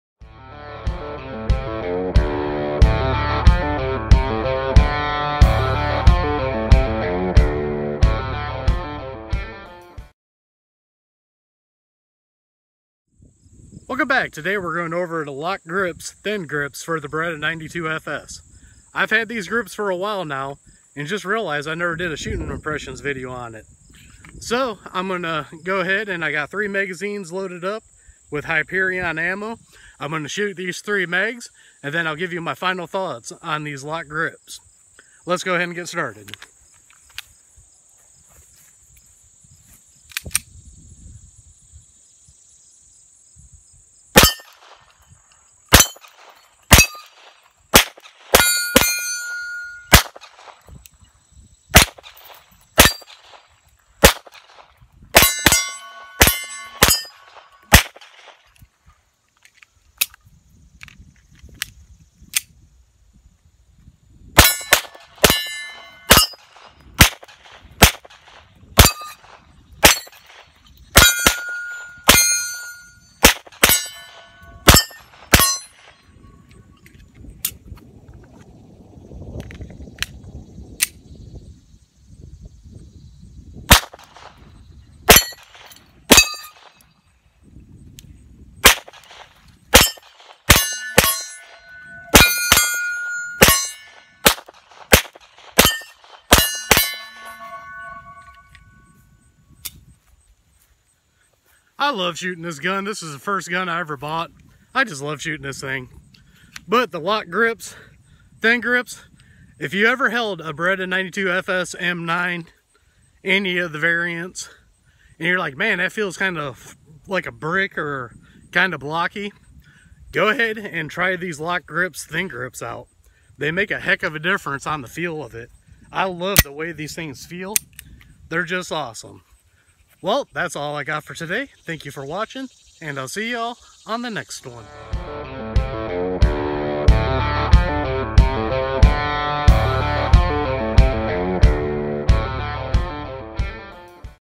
Thin Lok Grips Beretta 92fs (shooting impressions)
The thin grips make the beretta 92fs feel so much better in the hand.

Amazon Storefront